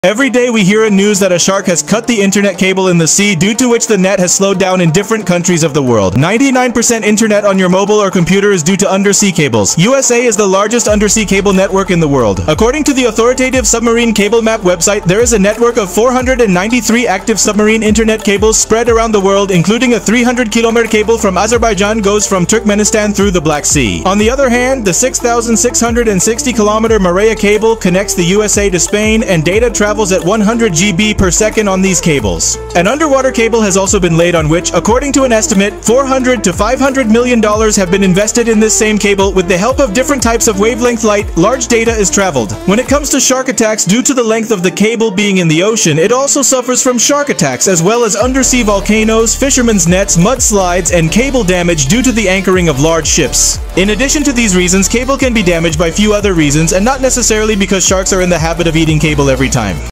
Sharks vs. Undersea Cables | Unveiling the Mystery of Undersea Internet Cables
Every day, we hear news of sharks cutting undersea internet cables, slowing down the net in various countries. Did you know that 99% of your mobile or computer internet comes from these undersea cables? The USA boasts the largest undersea cable network in the world. According to the authoritative submarine cable map website, there are 493 active submarine internet cables globally. One notable cable is a 300 km link from Azerbaijan to Turkmenistan through the Black Sea, while the 6,660 km Marea cable connects the USA to Spain, transmitting data at 100 GB per second.
Undersea cables, which cost an estimated $400 to $500 million each, use various wavelengths of light to carry large amounts of data. Besides shark attacks, these cables face threats from undersea volcanoes, fishermen's nets, mudslides, and large ships' anchors. While sharks do cause damage, they aren't the sole culprits in disrupting our underwater internet highways.